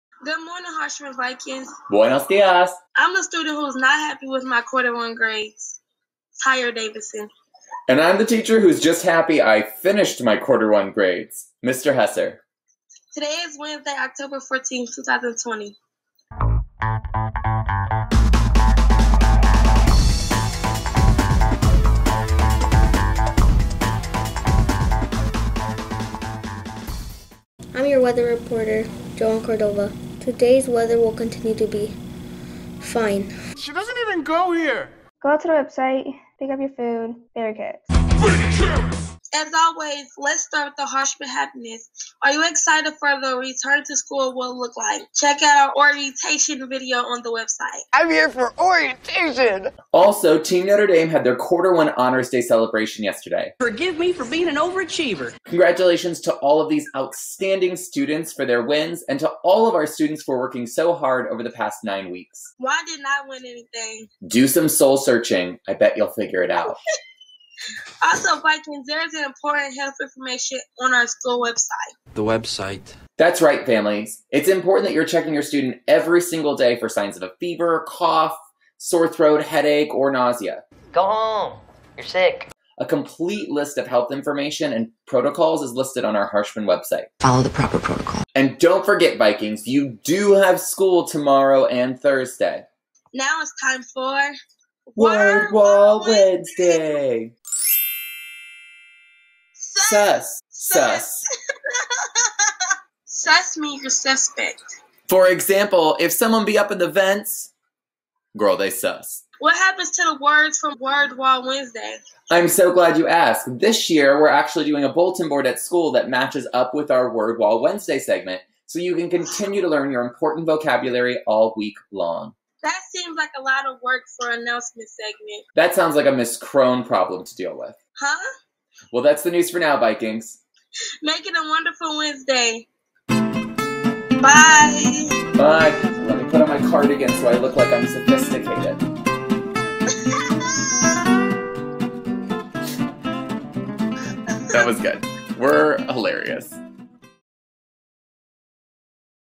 Daily Announcements: Wednesday, October 14th, 2020
An unorthodox word of the week, and a friendly reminder that students DOHAVE SCHOOL THURSDAY AND FRIDAY.